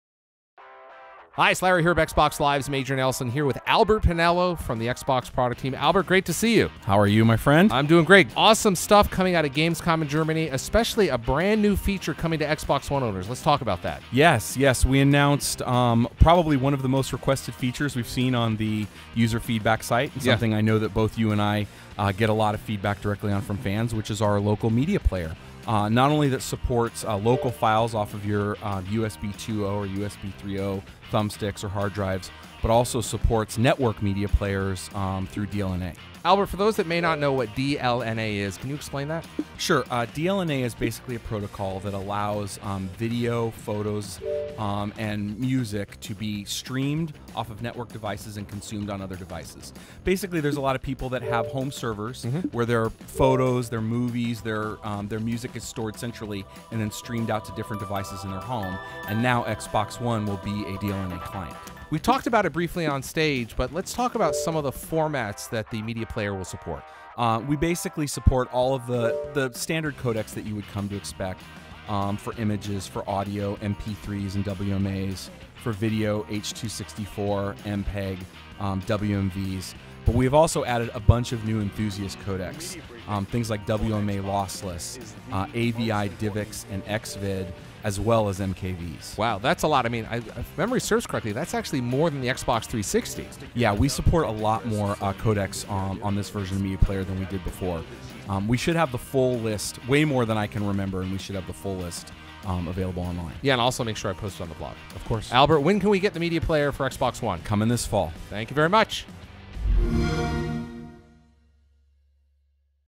Xbox One - Media Player Preview [EN]
Watch the new Xbox One - Media Player Preview. Larry Hryb, Xbox Live's Major Nelson chats with Albert Penello on the Xbox team about the upcoming Media Player, including DLNA support.

Thanks for all your support, rating the video and leaving a comment is always appreciated! Please respect each other in the comments!